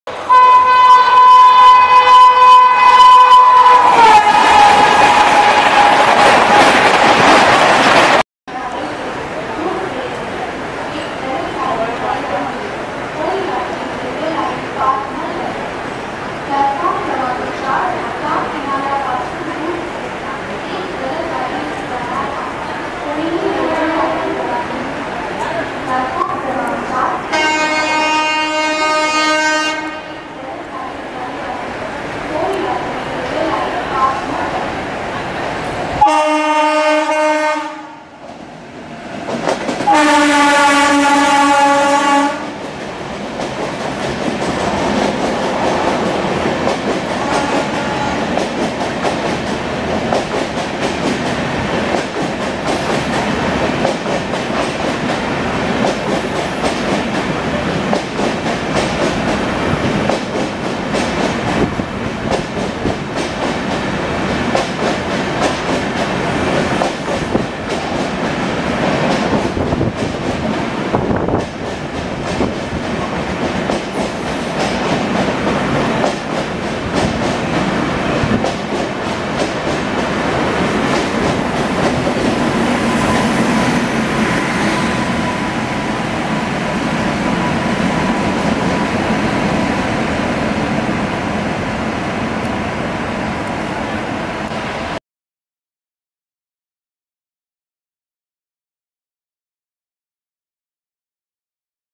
First Push-Pull Anniversary || Mission Raftaar Livery KYN WAP-7s power 22221 CST Rajdhani through DR
Tricolour painted , Swachh Bharat Liveried KYN HOG WAP-7s 30579 & 30580 in Push Pull mode skip Dadar with the 22221 Mumbai CSMT- Hazrat Nizamuddin Rajdhani Express.